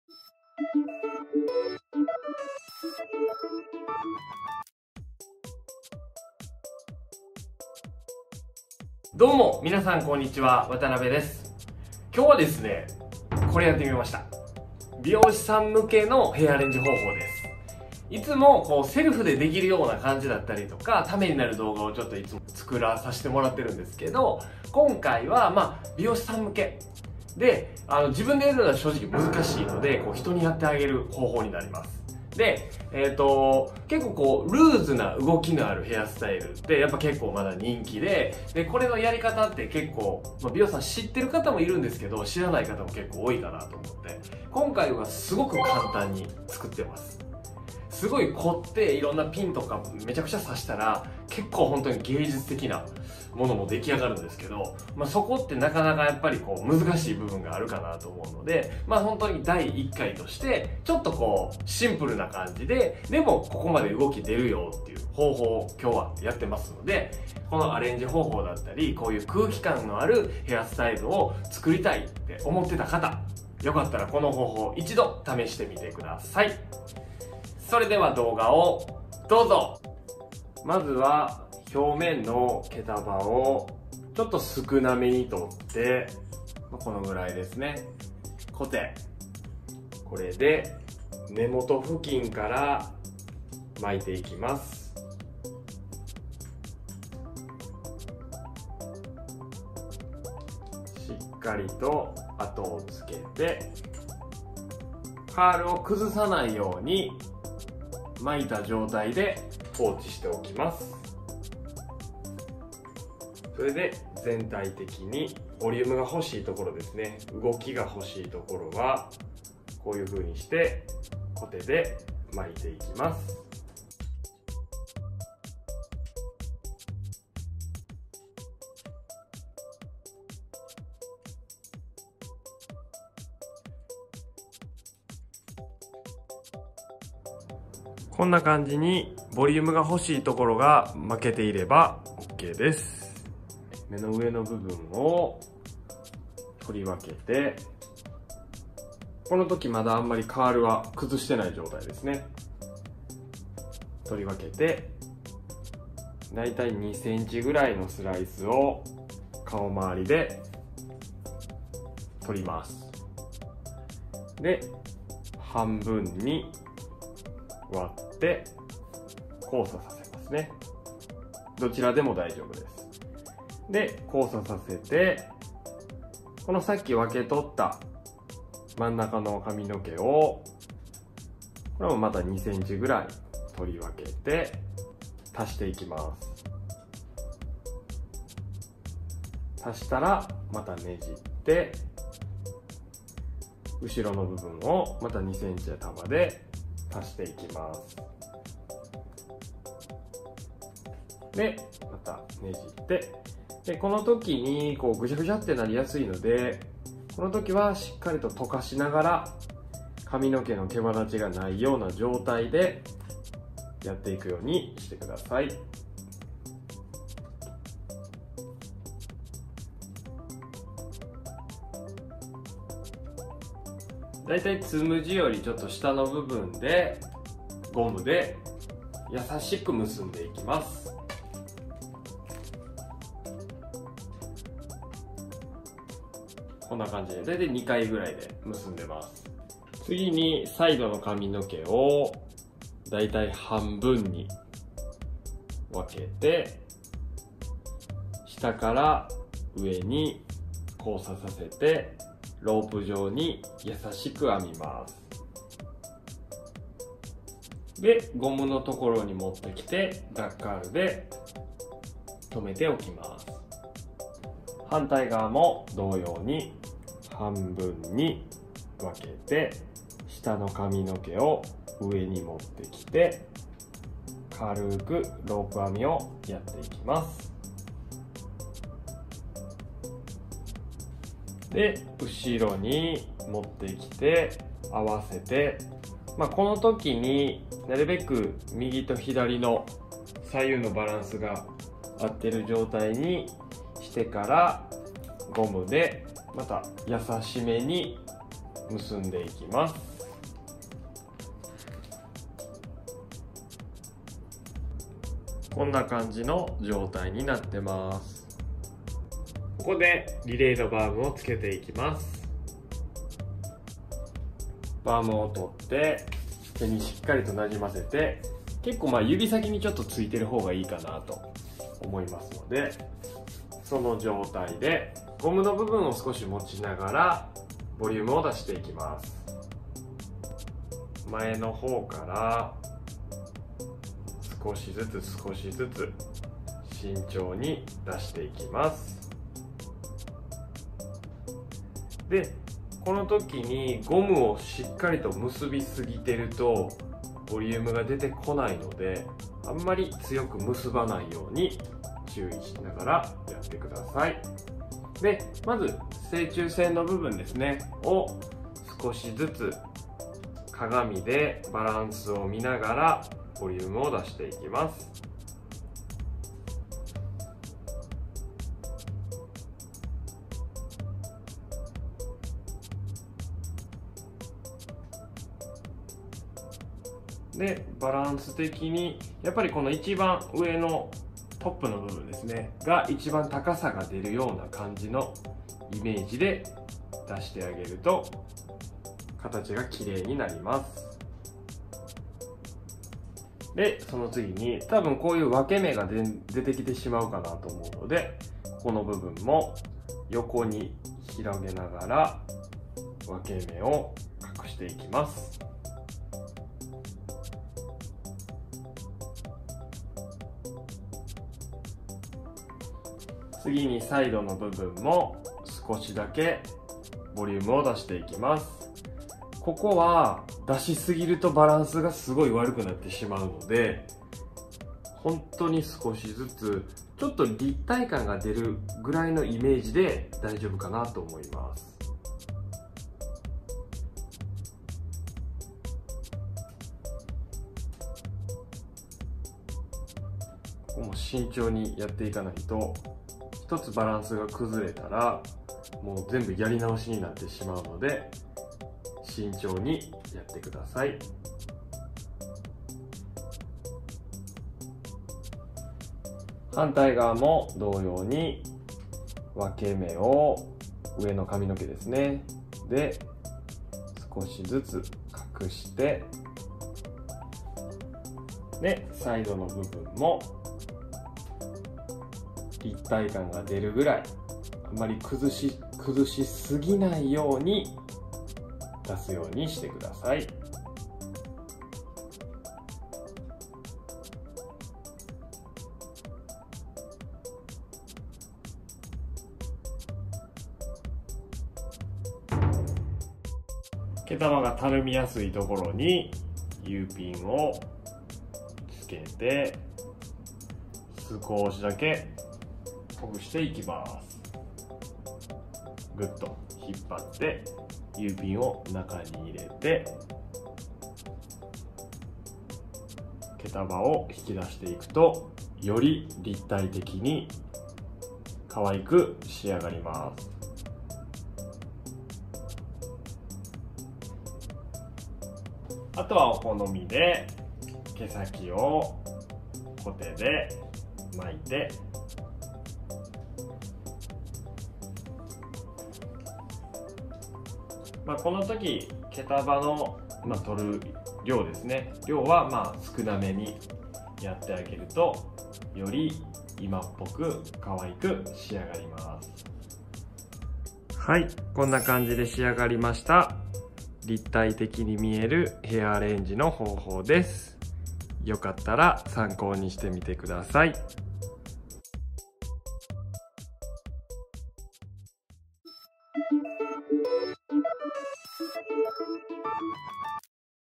特典あり！！美容師さん向け 動きのあるヘアアレンジの作り方♪ SALONTube 渡邊義明 Hair styling Hair arrangement 头发 헤어
美容師さん向け　動きのあるヘアアレンジの作り方を

すごく簡単に作っています！
お客さんにこうゆうアレンジを求められた時に
参考にしてみてください！

【使ったもの】
・アイビル25㎜

この動画だけの特典　LILAY Free Balm　10%OFFになります！

・Uピン

美容動画

Hair styling
Hair arrangement
头发
헤어